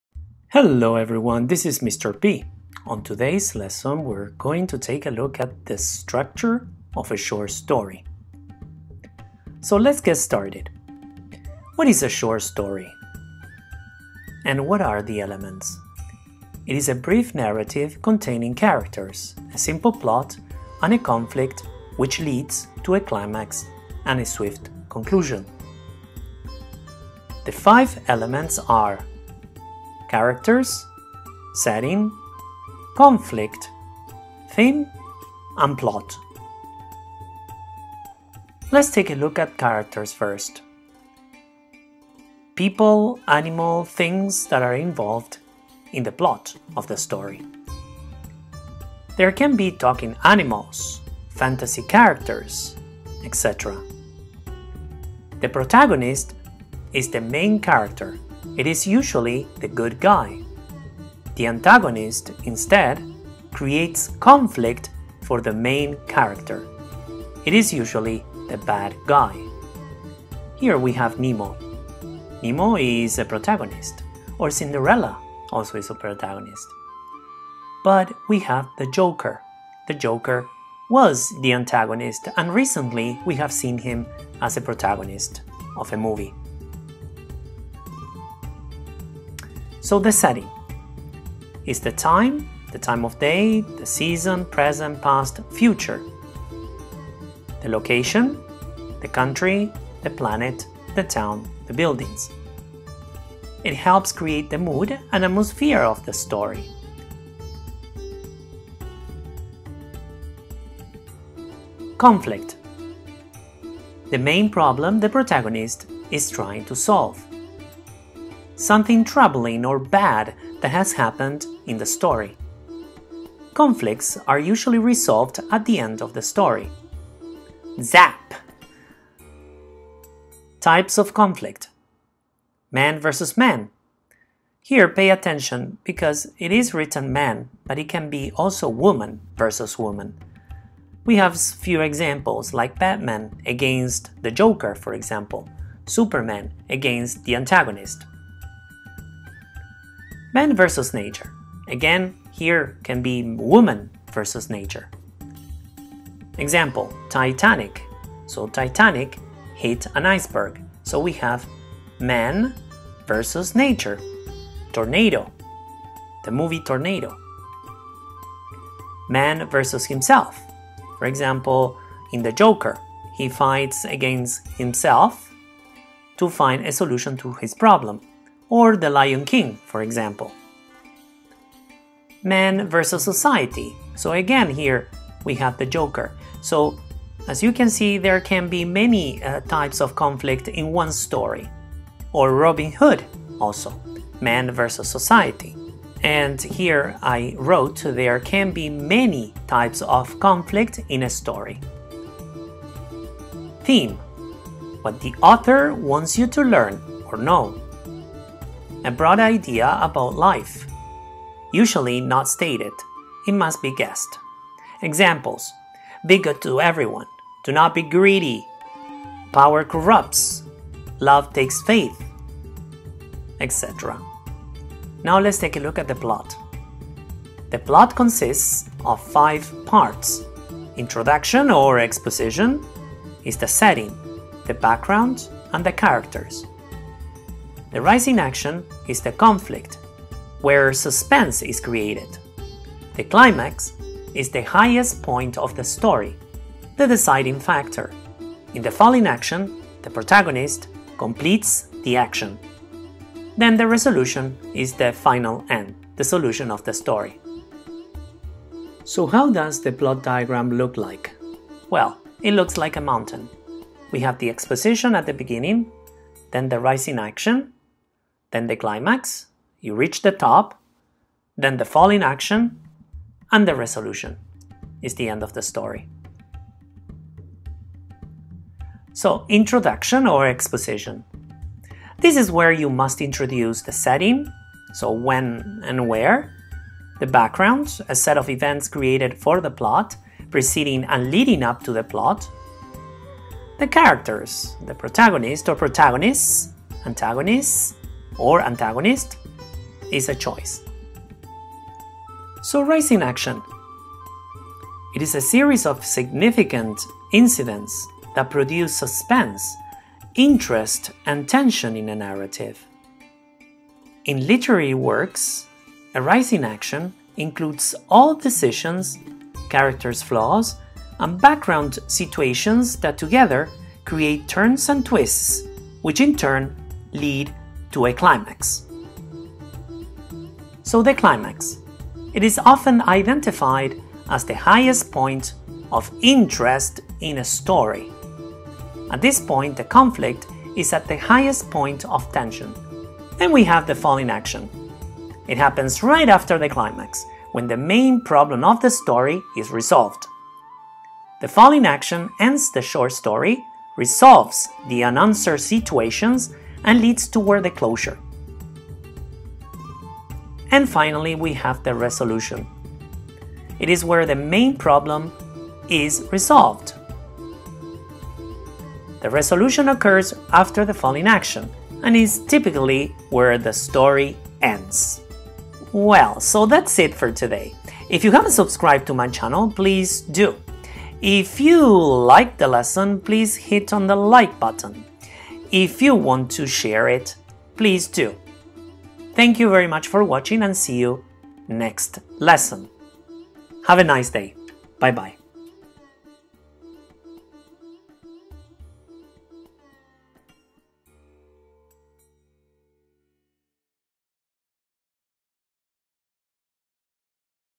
The Structure of a Short Story including plot diagram
What is a short story? and What do we need to write one?. These are some of the questions Mr. P. will answer regarding short stories. He will first start the lesson by introducing the elements of a short story. Then, he will focus on the structure, and thus how and what you need in order to write and a short story. At the end of the video, he will delve into the plot diagram and its different parts.
This lesson is designed for intermediate students who wish to improve their English grammar, and for those above levels who wish to clear up doubts about this topic.